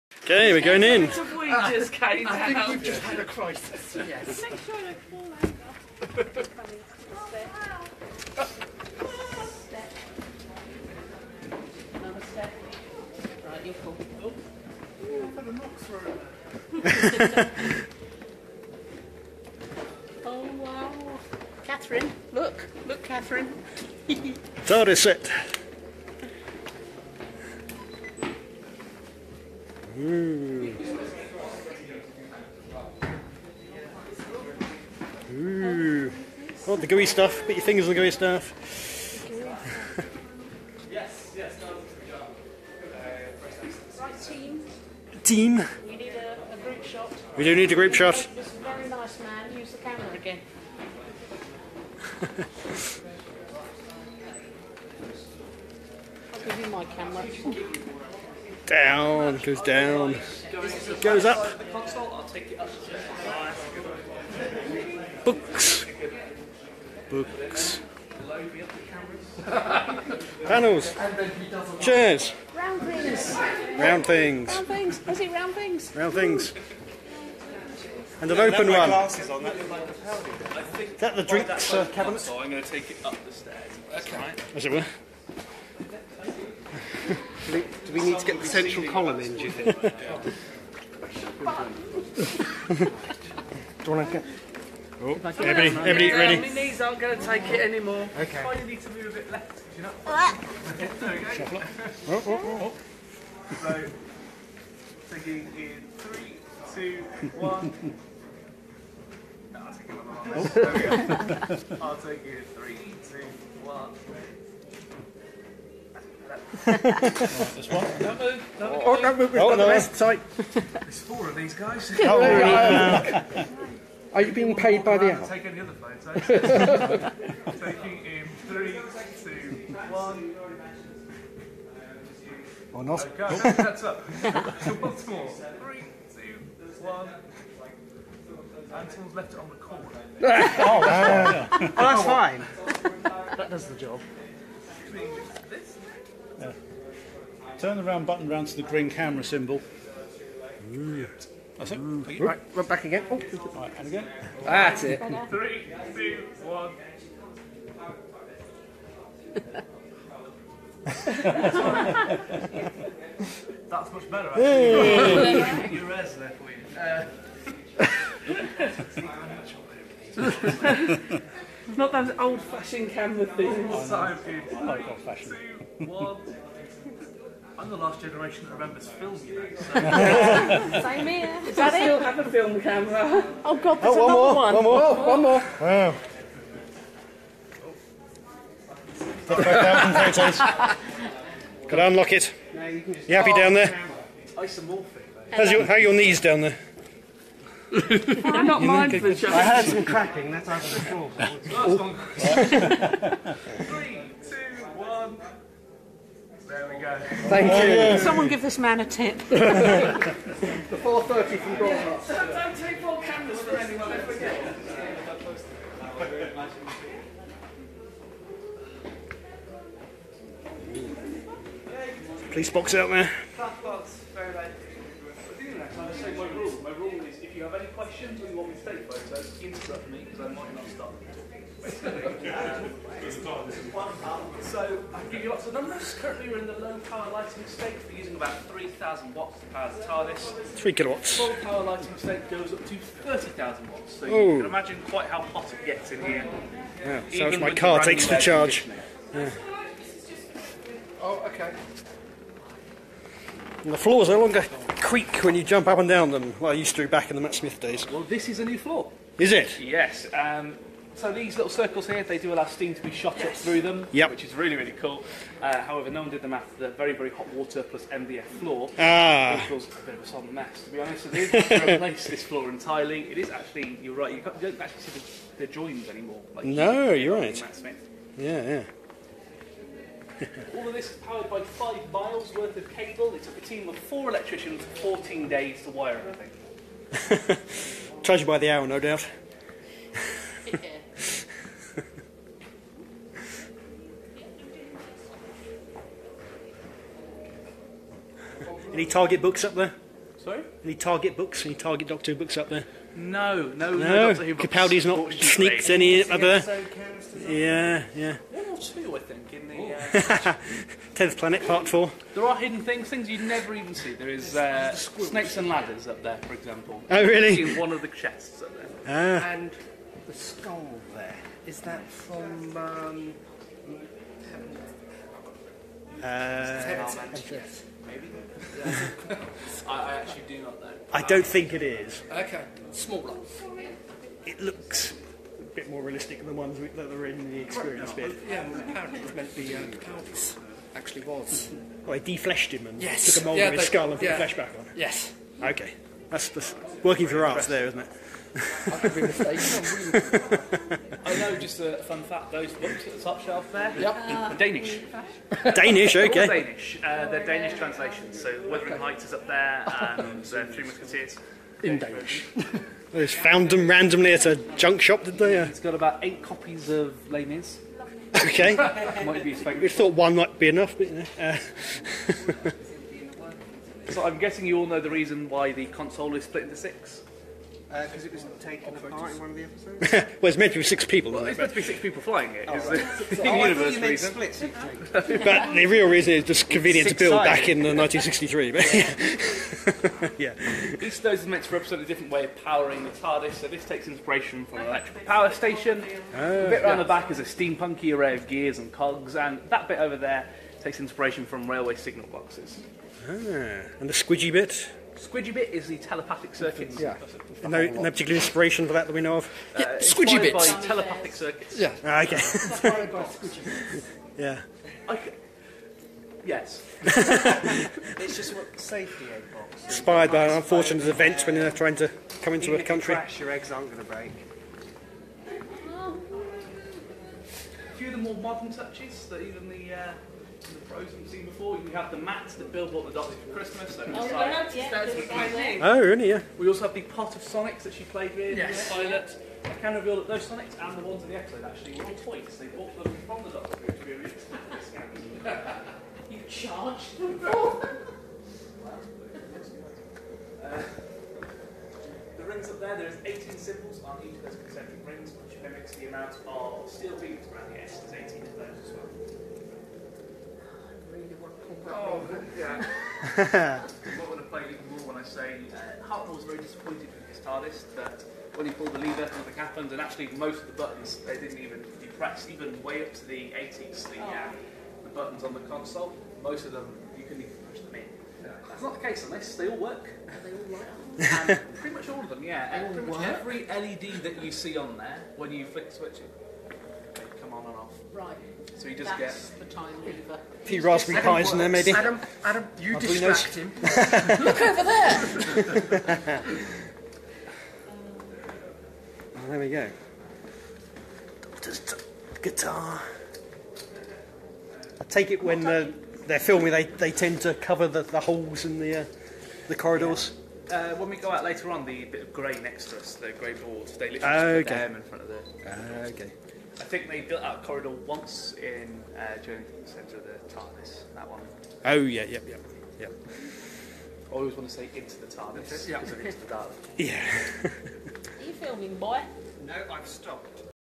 Doctor Who TARDIS Set Tour, May 2017
The Staggering Stories team have a guided tour of the Doctor Who studio in Roath Lock, Cardiff Bay.